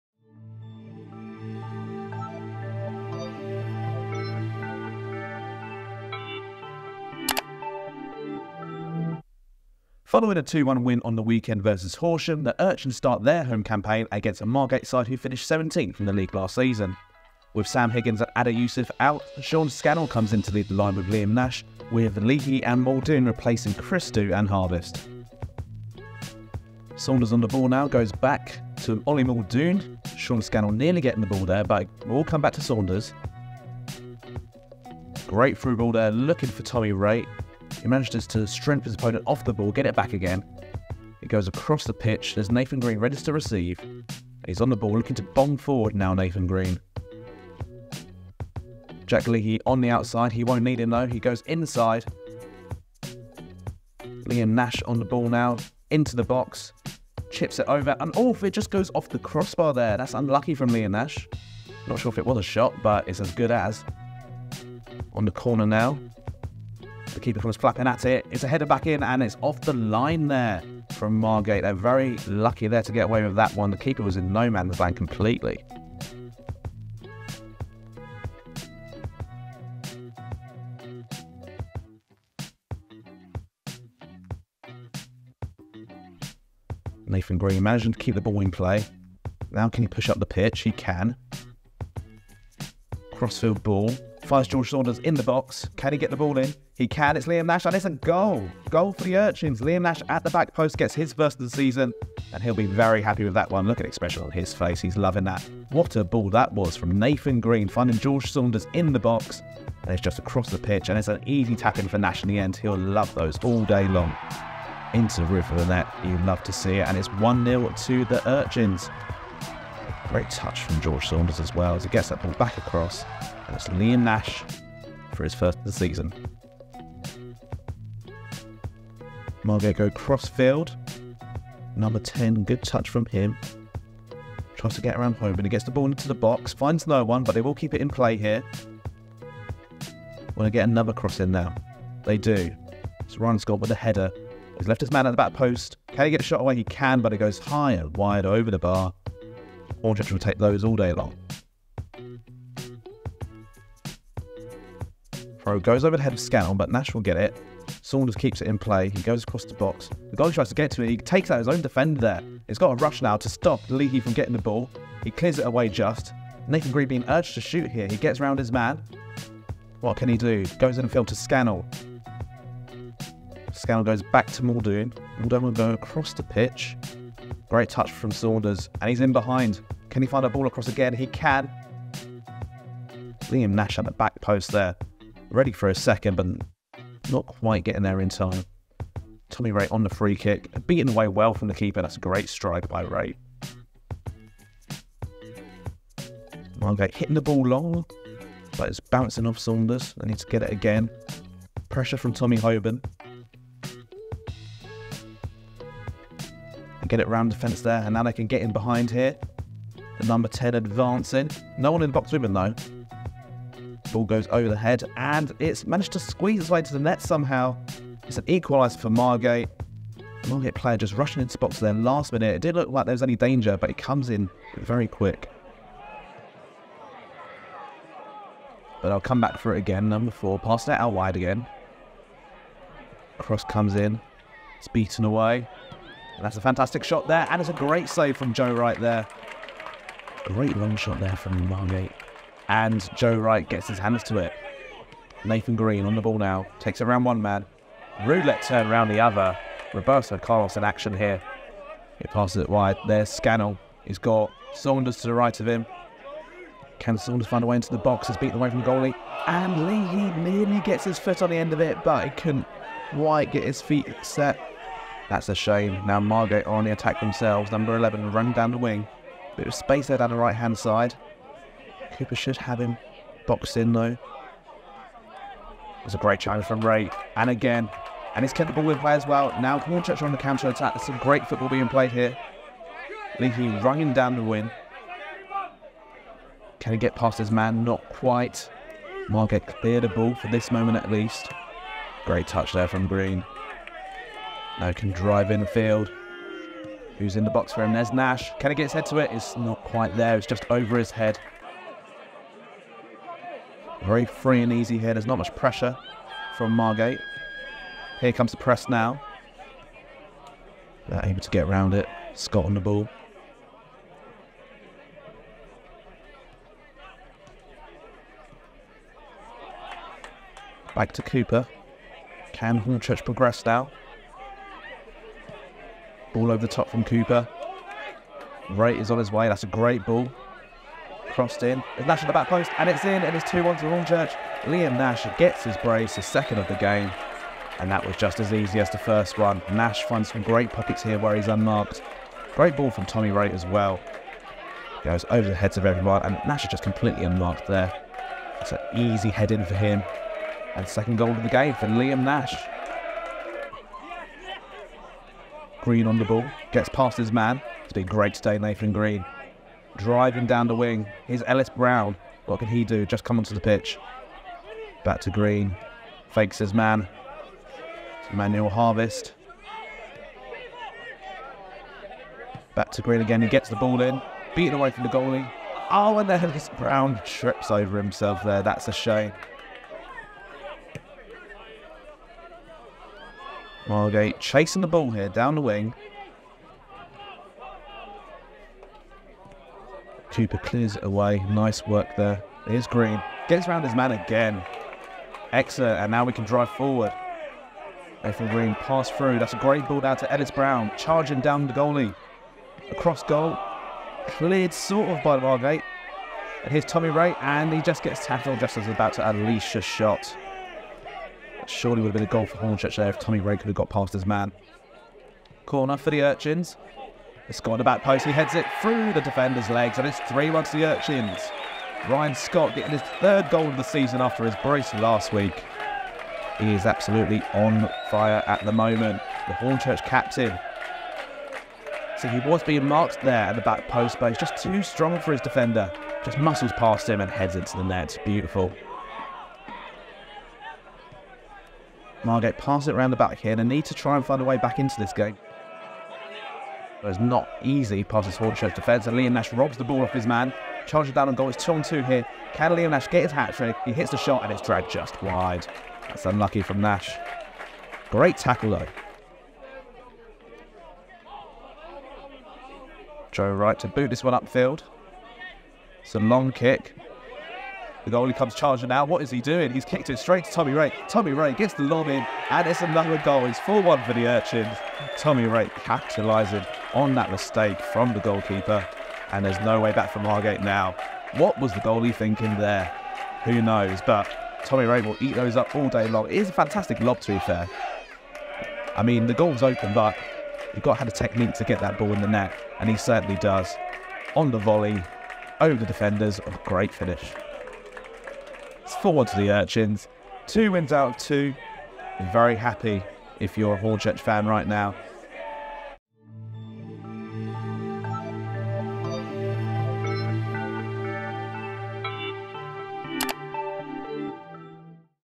RESOUNDING VICTORY! | Hornchurch 4-1 Margate | Isthmian Premier League 2023/24
Hornchurch made it two wins in two to start the season thanks to goals from Tom Wraight, Ryan Scott and a brace from Liam Nash.

Filmed, edited and commentary by Billy Taylor / BTT Visuals.